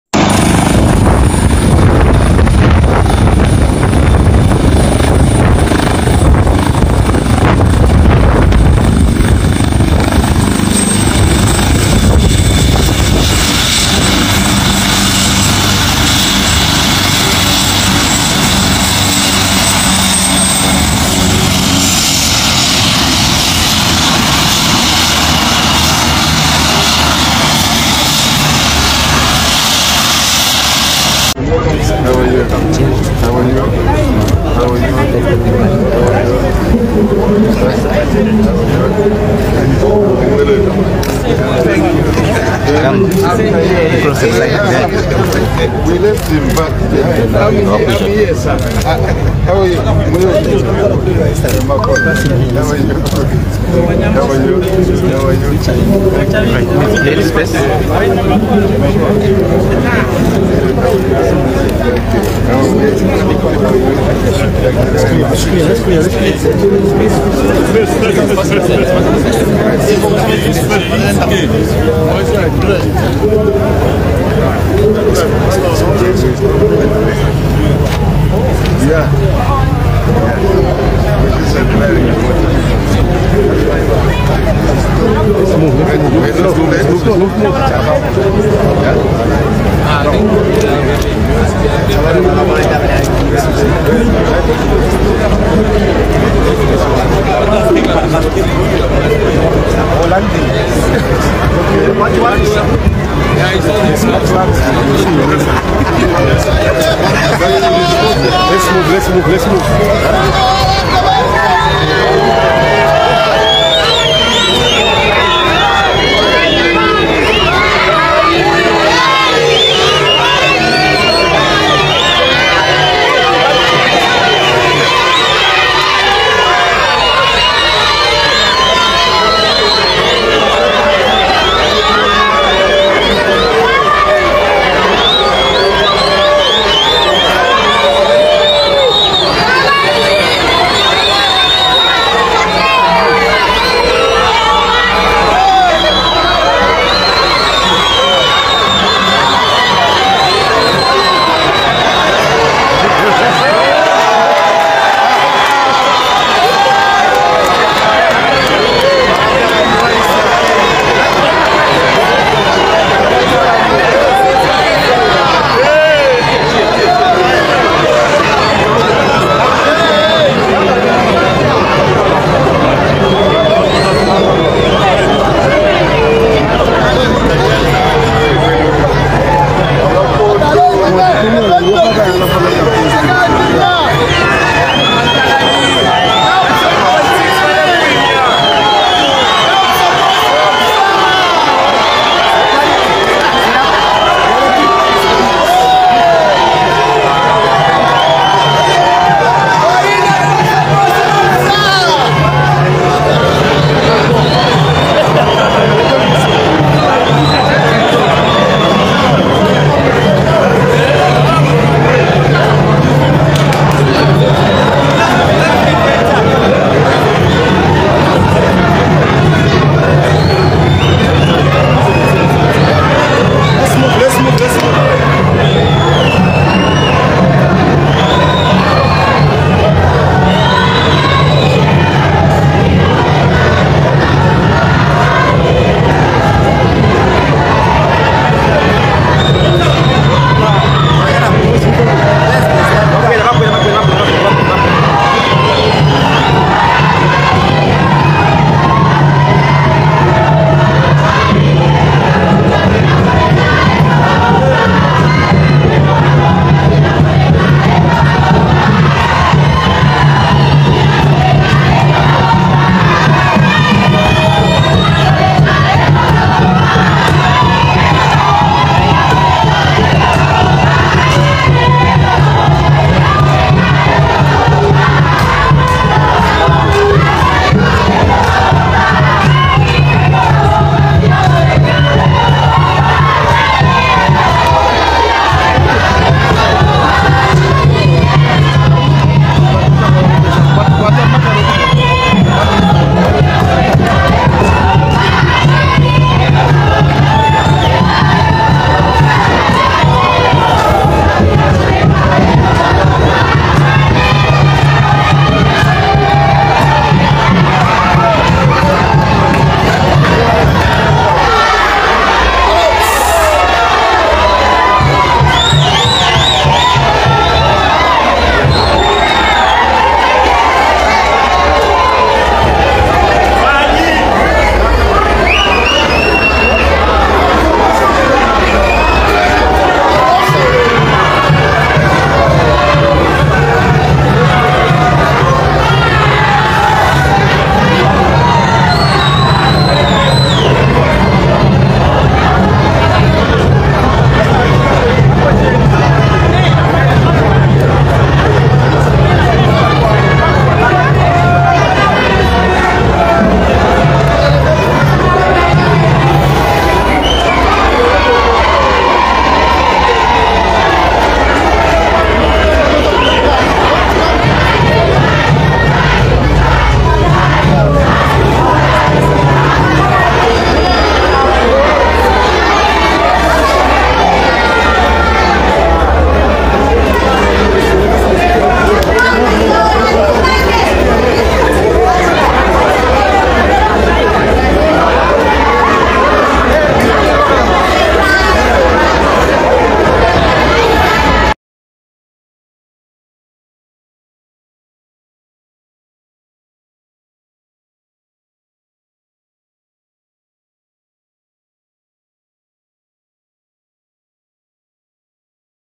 Super Kena receives President HH in Ndola
Hakainde Hichilema
President of the Republic of Zambia.
Arriving at Levy Mwanawansa Stadium in Ndola,  Copperbelt province to witness the second edition of the Confederation of African Athletics.  @ZedHighlightsTv.